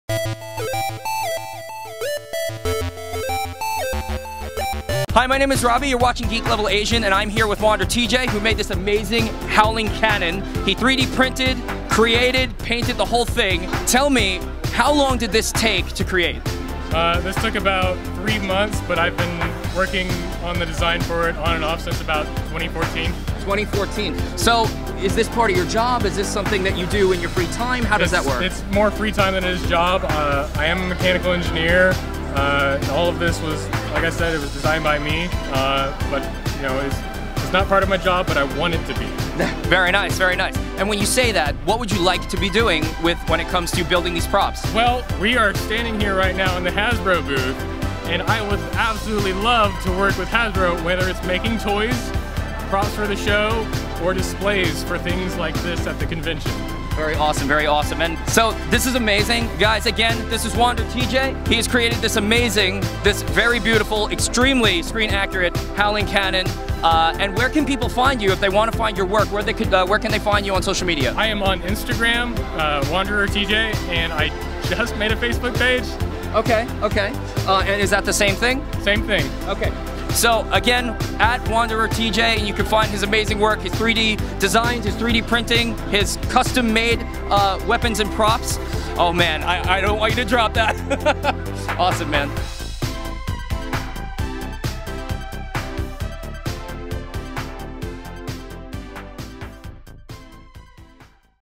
POWER MORPHICON 2018 WandererTJ Howling Cannon Interview
Behold, the Howling Cannon!  My whole life I've wanted to see this work of art in real life, and thank you to Tanner Jones (WandererTJ), that dream became a reality.

Tanner is a mechanical engineer who is a Power Rangers fan at heart and utilized his talents and education for this one of a kind passion project that was turning quite a few heads at Power Morphicon 2018.

I learned that in the original show, the weapons were screwed together in order to stay in place.  Tanner took the same approach to make sure that the weapons were secure.  He also filled me in on some of the tv magic to help me understand that the widths of the handles for the Power Axe and and the Power Daggers are different once they're on the blaster.  But that was no problem for Tanner, who created separate handles to compensate for the widths.

If you would like to find his work online you can visit him on Instagram and Facebook by searching for WandererTJ.  I follow him and you should too!